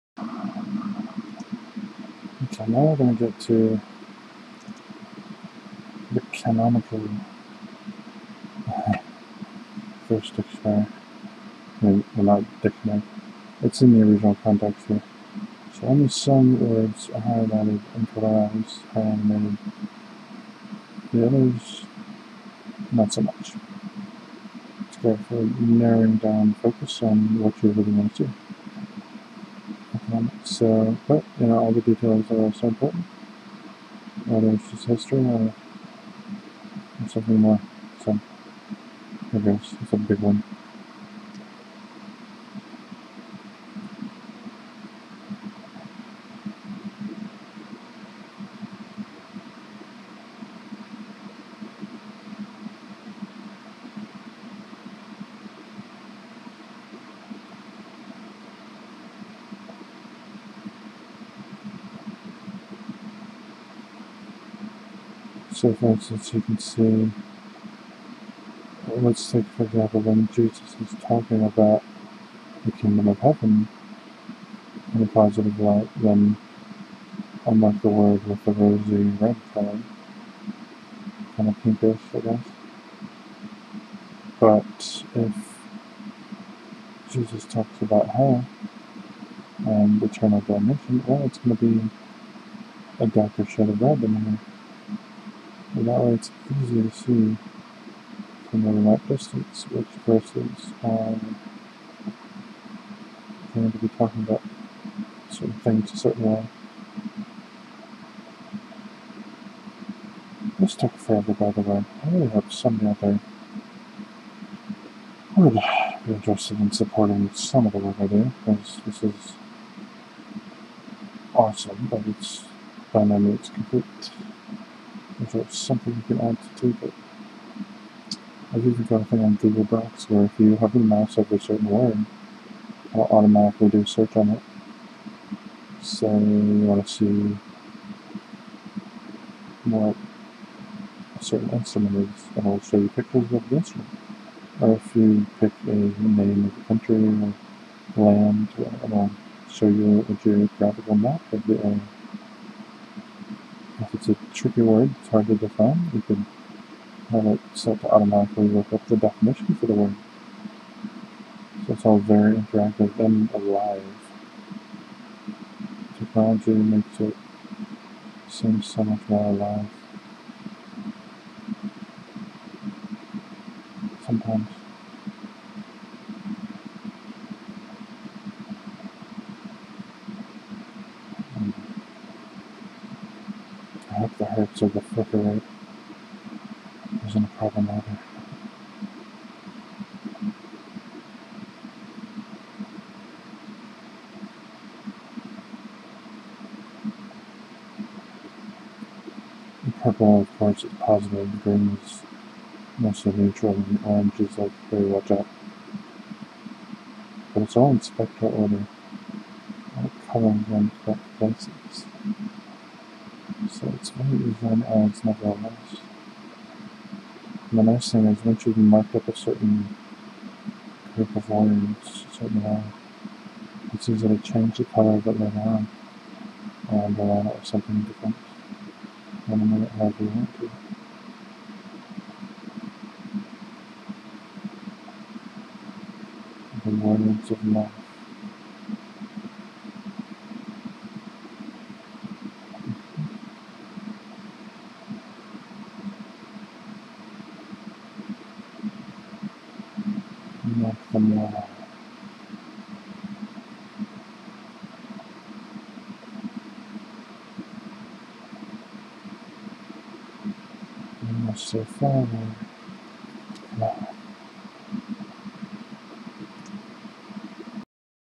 5 Canonical 1611 AKJV Old & New Testaments underline with rainbow color center verse & animation
Rainbow Holy Bible Verse Marking Project - colorized key favorite memorable verse collections - with underlines, boldface, highlighting, animations, italics, etc. - made with the help of E-Sword of the Lord computer software program...